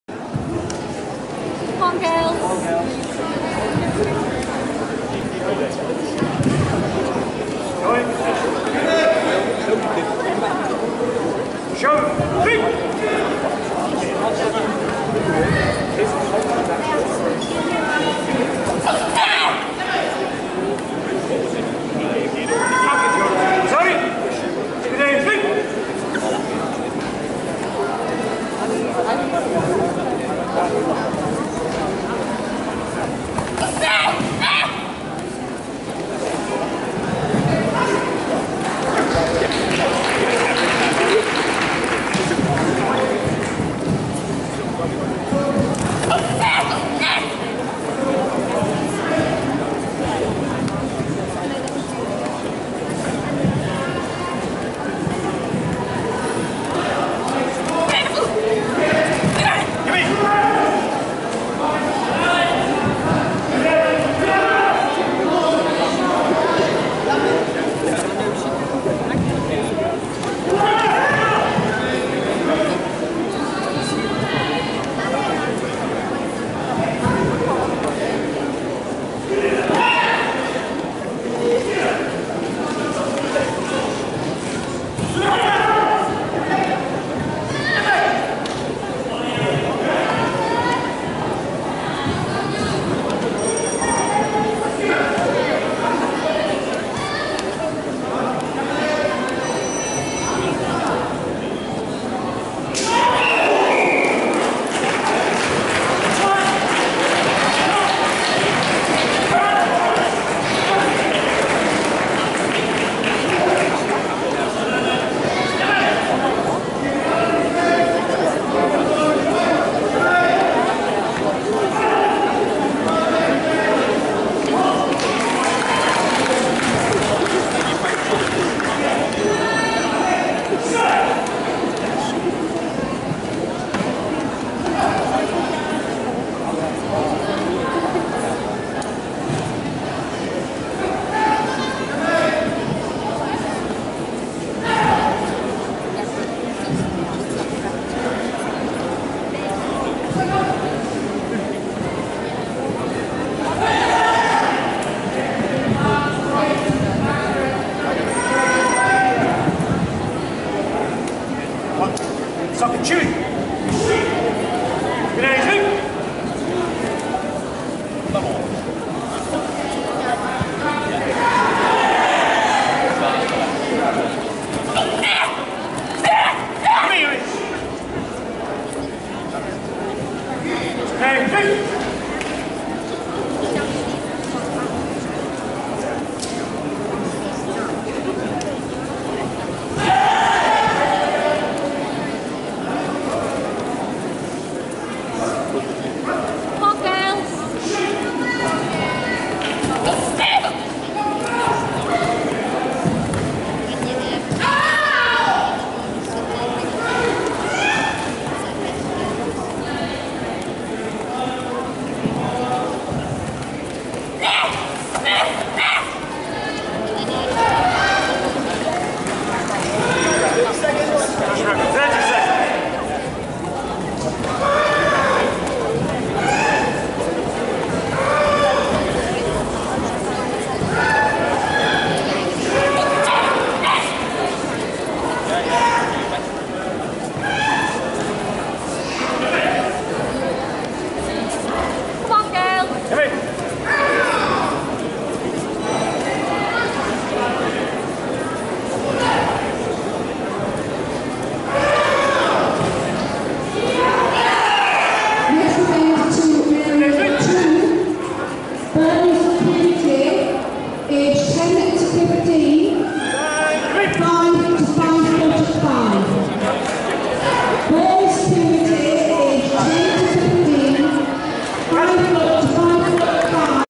Laura Bunnett V Summer Groves Girls Kumite
Northern Region Championships
University of Sunderland
Saturday 21st October 2017.